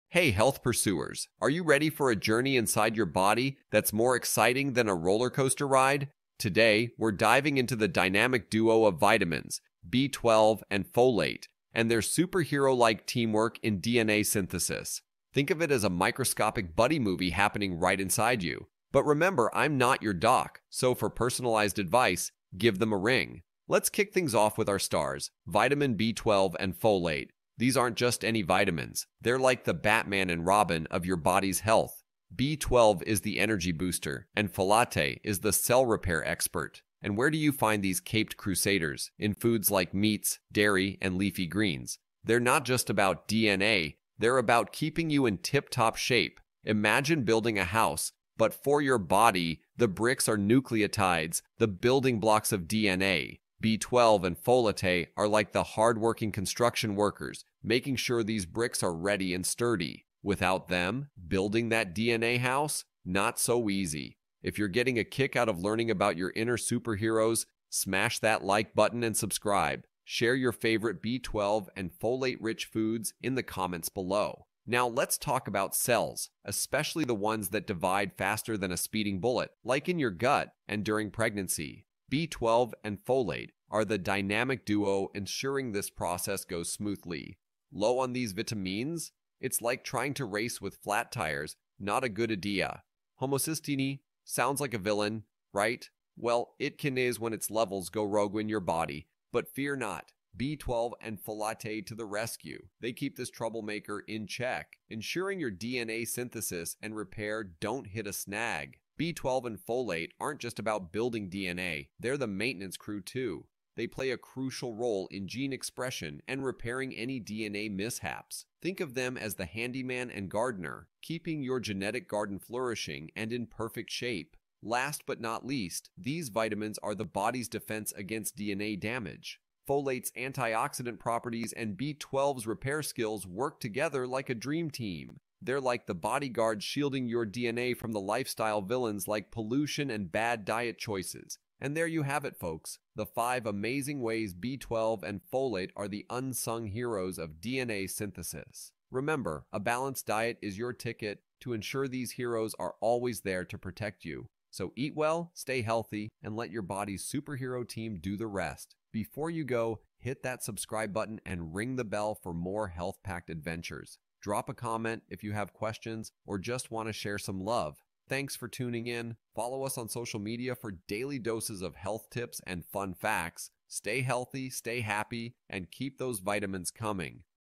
5 Ways B12 and Folate Team Up for DNA Synthesis
Join us on an exhilarating journey inside your body with this health-packed adventure exploring the dynamic duo of vitamins – B12 and Folate. In this video, we delve into the superhero-like roles these essential nutrients play in DNA synthesis, cell repair, and overall wellness.

🦸‍♂️ Discover Your Inner Superheroes: Learn how Vitamin B12 and Folate, akin to Batman and Robin, team up to boost your energy, repair cells, and ensure smooth DNA synthesis.

🥦 Find Your Nutrient Heroes in Food: We'll guide you through the best dietary sources of these vitamins, including meats, dairy, and leafy greens, and how they contribute to keeping you in top shape.

➡️Vitamin B12
➡️Folate
➡️DNA Synthesis
➡️Nutrition
➡️Healthy Diet
➡️Cell Repair
➡️Homocysteine
➡️Gene Expression
➡️Antioxidants
➡️Pregnancy Nutrition